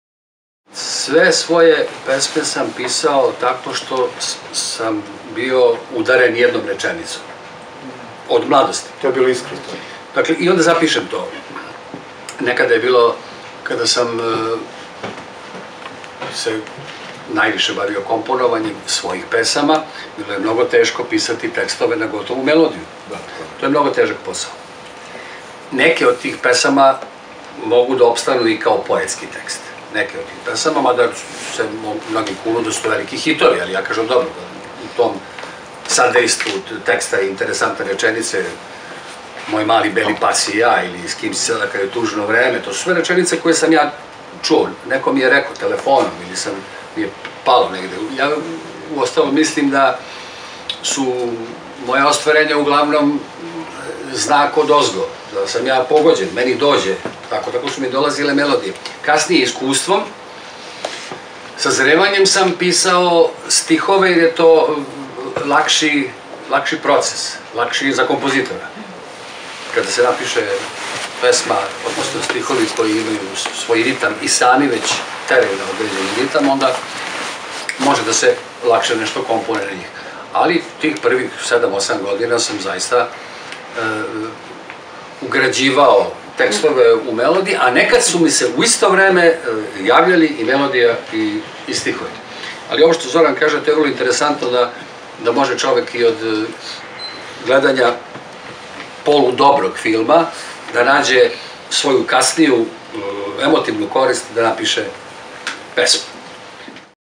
Boris Bizetić - O pisanju pesama
Na poetskoj večeri u biblioteci Milutin Bojić.